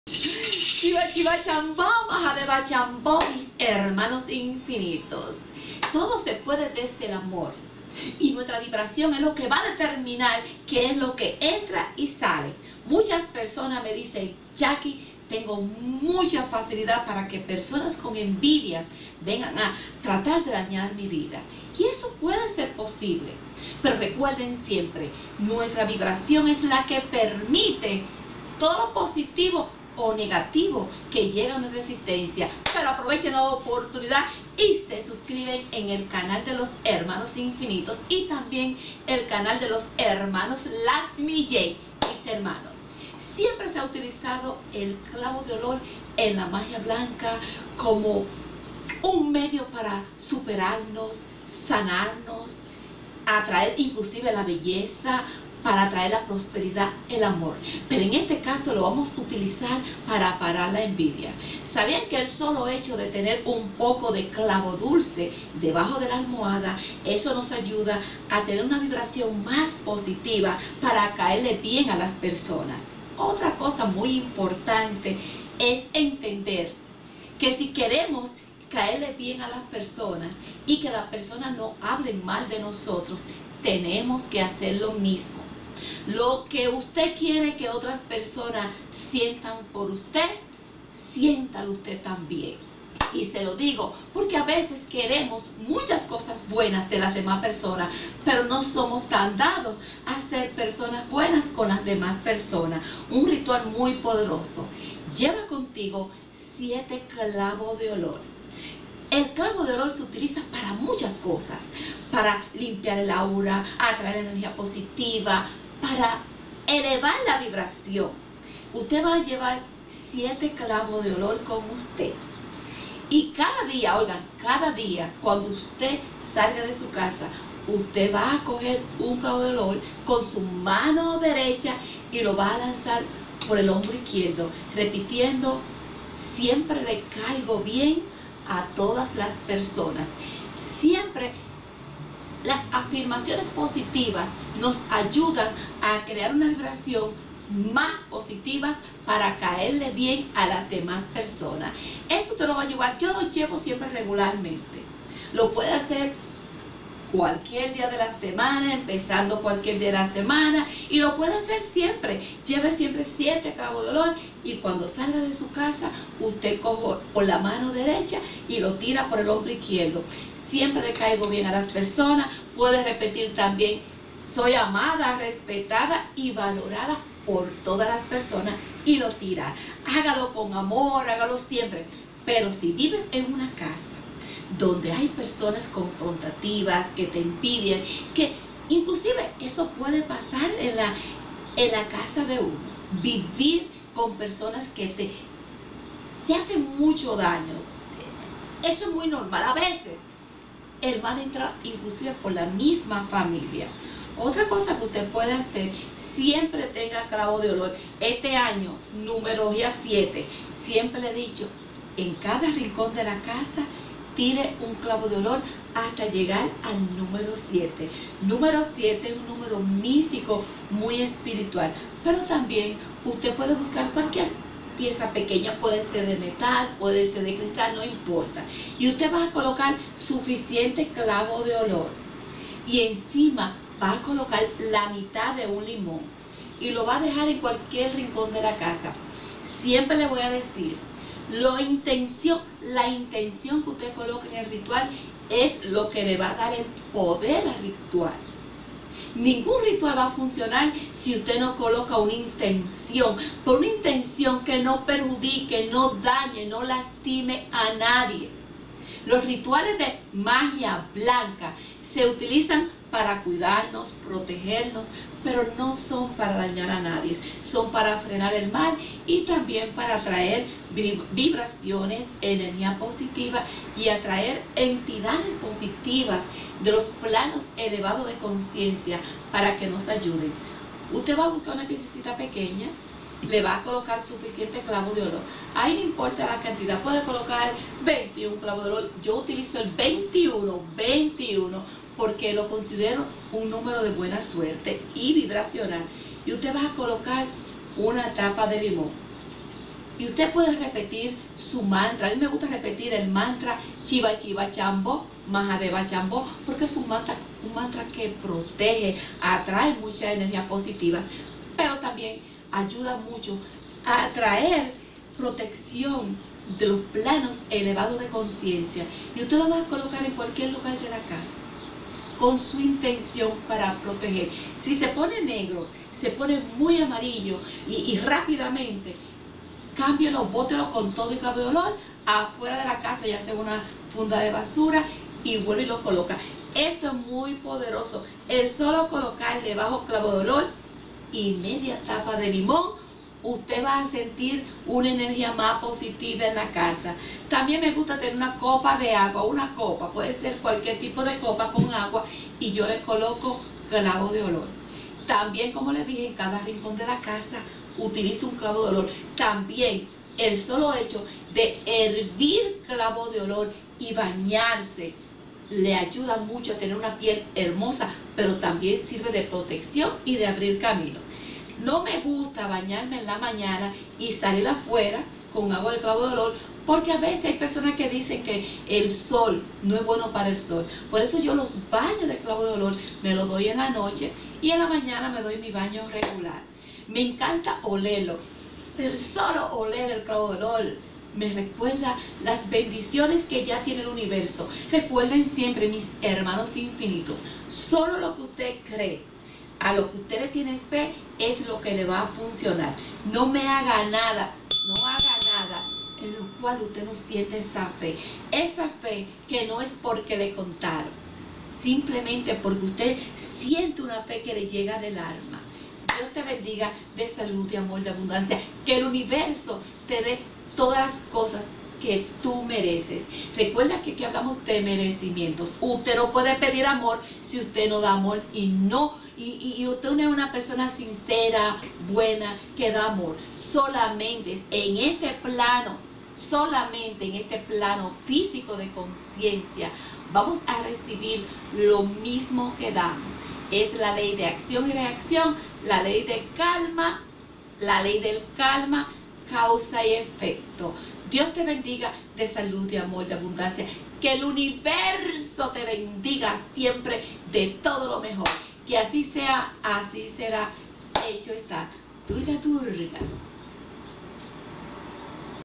Sabias que la ENVIDIA puede evitarse con el CLAVO de OLOR.
Sabias que la ENVIDIA puede evitarse con el CLAVO de OLOR, claro tu actitud puede marcar la diferencia, tus sentimientos y pensamientos hacia las personas pueden atraer buena o mala energía. Un simple cambio de actitud puede hacer mucho bien a tu vida y atraer gente de mucho valor.

Desde la antiguedad el clavo de olor se ha utilizado para la magia blanca en rituales de amor, atracción, purificación y para la prosperidad.

Mágicas bendiciones.